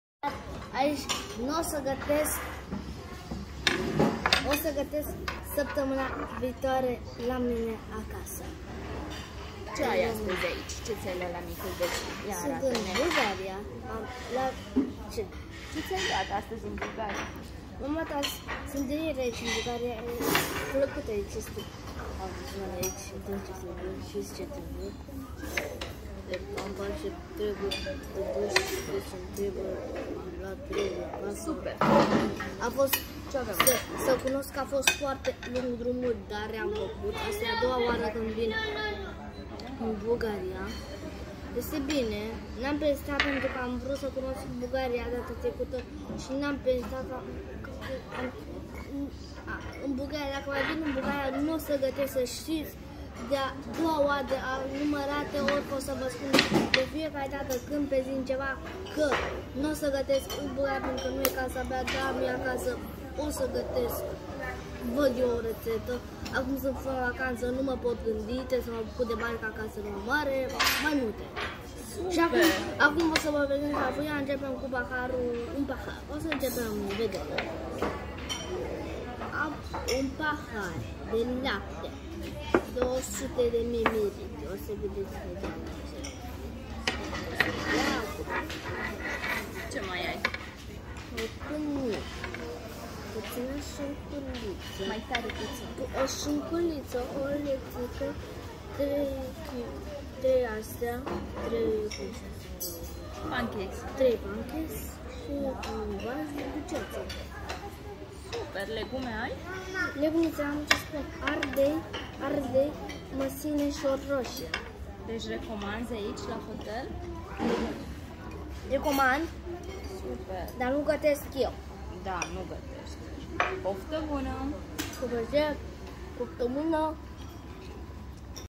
Sergiu in vacanta! 🏊‍♂️🌊🌞
Sergiu nu gateste in vacanta, dar va prezinta bucuros ce mananca!
Aici avem micul dejun! 🥞🥚🥛🍳